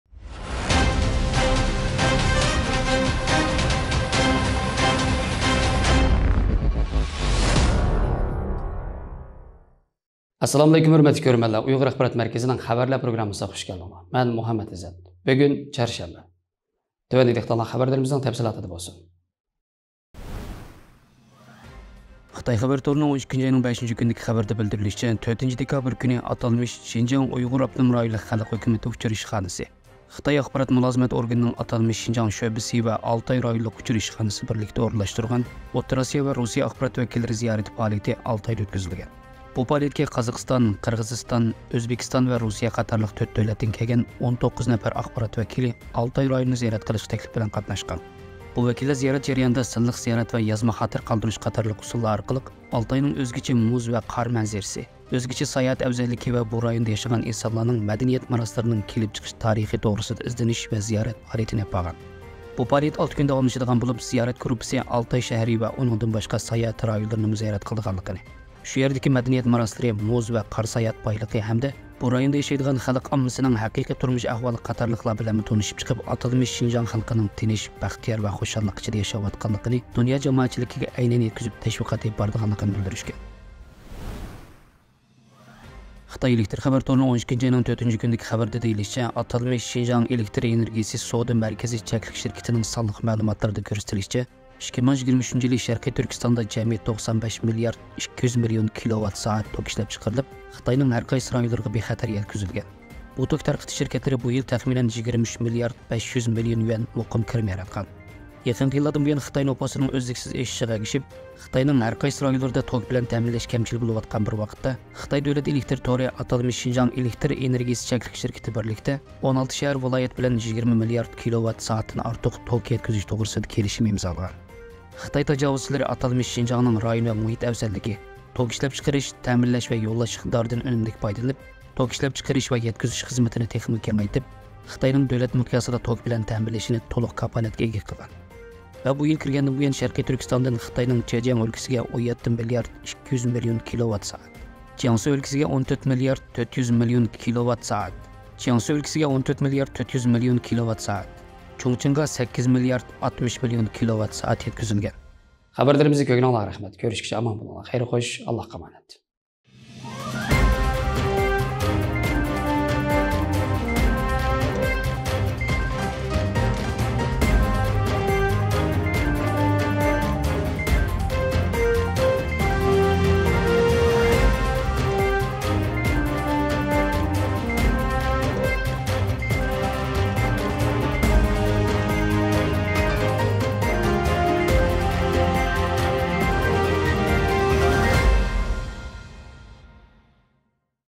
ئۇيغۇر ئاخبارات مەركىزى [UIC-News23.12.06] Ottura Asiya Axbarat Wekilliri Altaygha Ziyaretke Keldi
باش پىلانلىغۇچى : ئەركىن سىدىق
فىلىم مەسئۇلى: ئۇيغۇر كۆكئارت
فىلىم ئىشلىگۈچى : ئالپ ئارسلان

***بۈگۈنكى خەۋەرلەر***

ئوتتۇرا ئاسىيا ۋە روسىيەنىڭ ئاخبارات ۋەكىللىرى ئالتايغا زىيارەتكە كەلدى

شەرقىي تۈركىستان 2023-يىلى جەمئىي 95 مىليارد 200 مىليون كىلوۋات سائەت توك ئىشلەپ چىقارغان

تەييارلىغۇچى: ئالپ ئارسلان
ئاۋازدا: مۇھەممەت ئىززەت

ئۇيغۇر ئاخبارات مەركىزى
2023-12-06
Bash Pilanlighuchi : Erkin Sidiq
Filim Mes'uli : Uyghur Kök'art
Filim Ishligüchi : Alp Arslan

***Bügünki Xewerler***

Ottura asiya we rosiyening axbarat wekilliri altaygha ziyaretke keldi

Sherqiy türkistan 2023-yili jemiy 95 milyard 200 milyon kilowat sa'et tok ishlep chiqarghan

Teyyarlighuchi : Alp Arslan
 Awazda : Muhemmet Izzet

Uyghur Axbarat Merkizi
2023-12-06